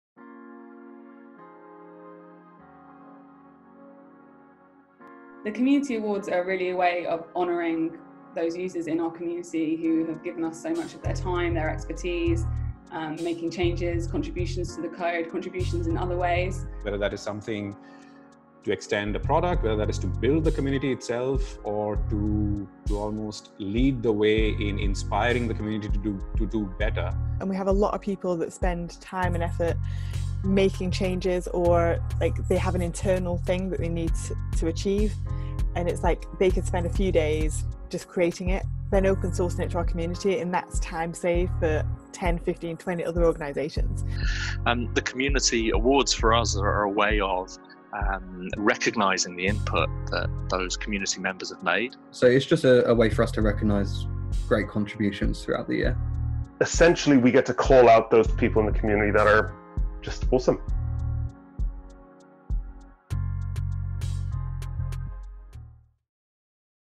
What are the Tyk Community Awards?
The Tyk Community Awards are our way of honouring those within our community who made contributions to API adoption.

Whether it’s code contributions, community blog posts, or new features: without these projects, Tyk wouldn’t be much use.  So these awards are our way of saying a huge thank you to everyone playing their part to kick down the barriers to API adoption.